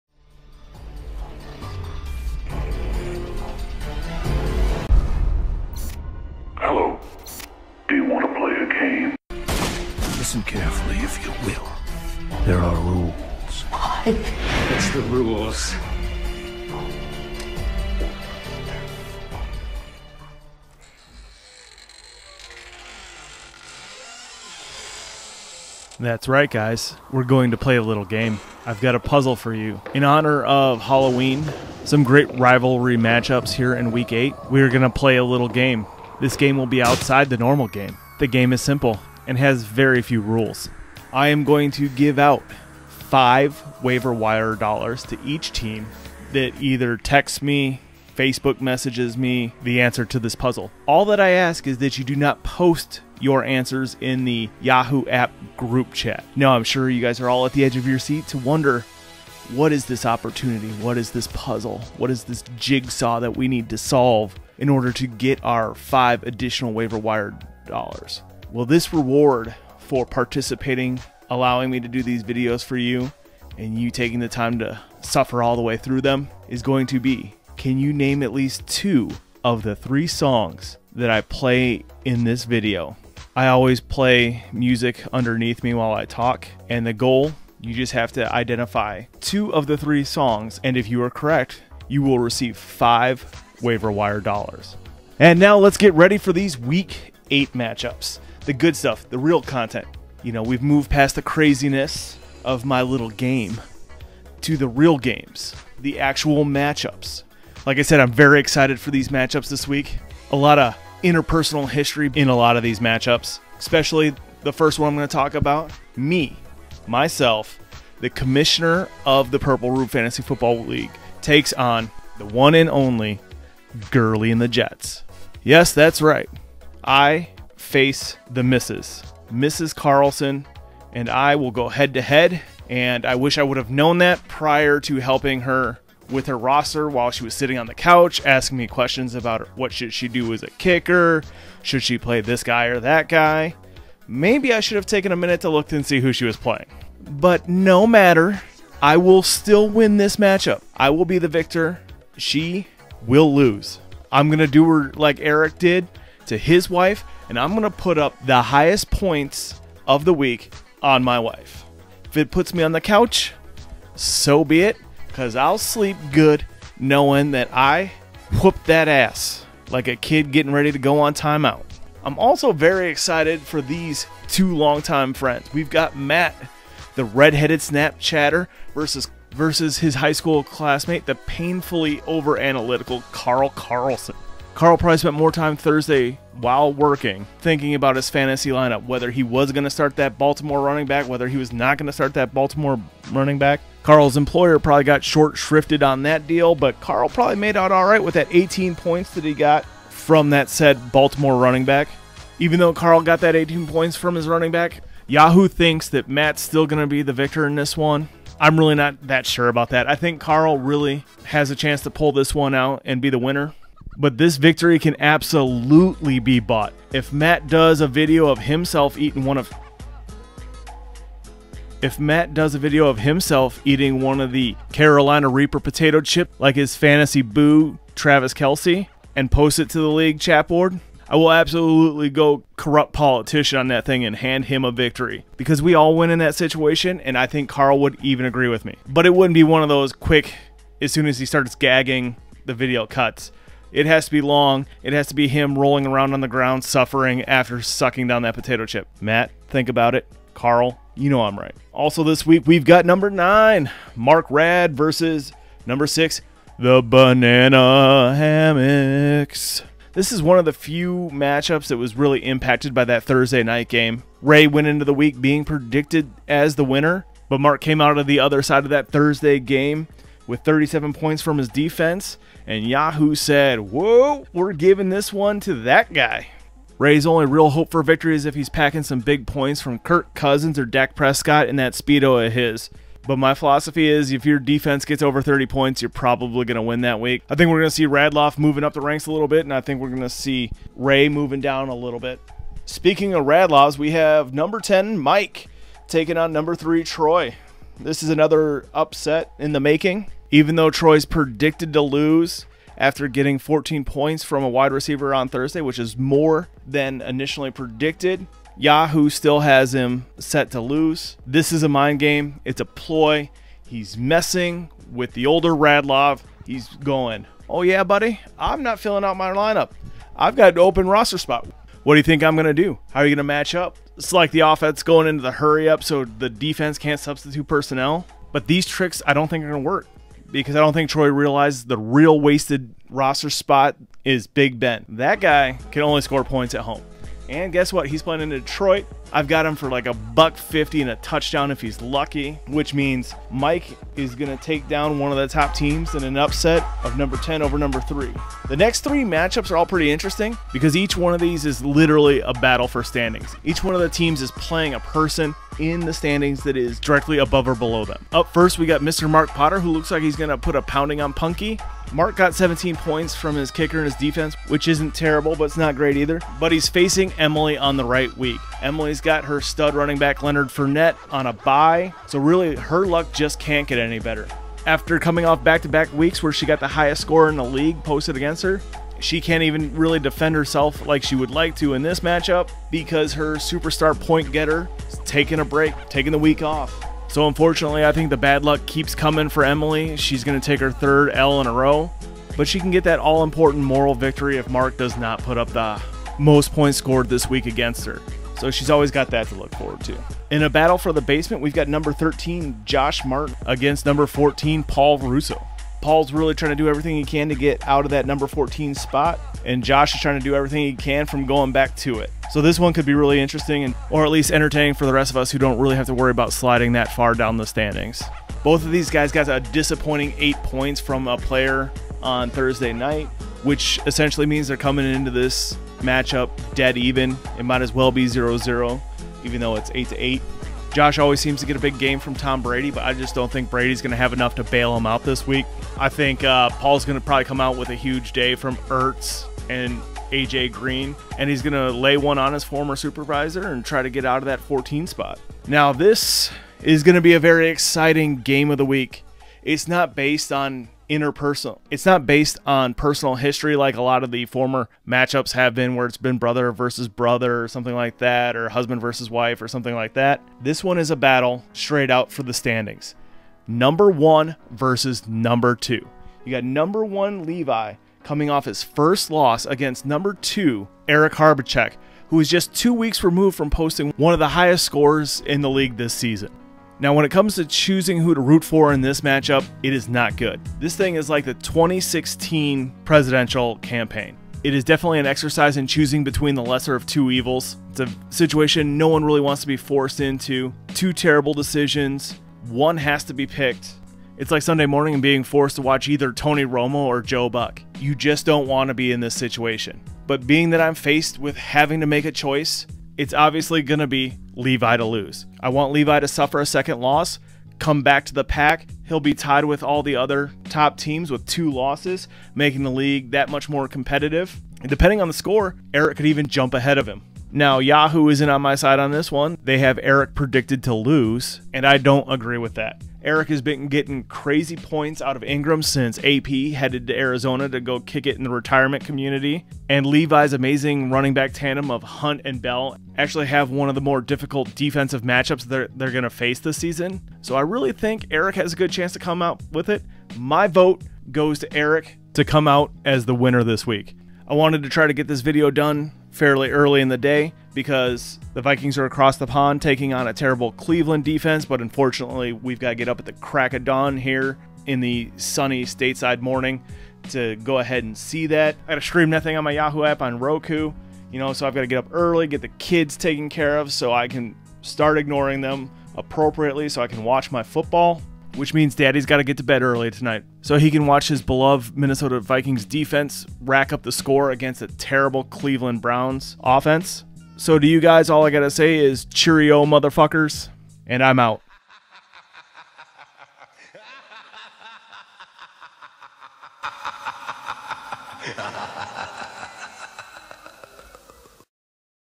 Week 8 preview Purple Rube
A look at the match ups coming up in week 8 for the Purple Rube Fantasy Football league. Plus a little game to engage the league members.